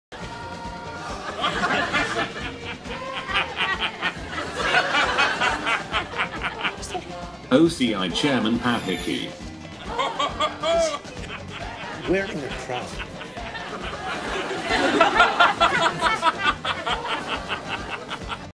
Having the craic in Rio
…Footage obtained of OCI Chairman Pat Hickey having the lolz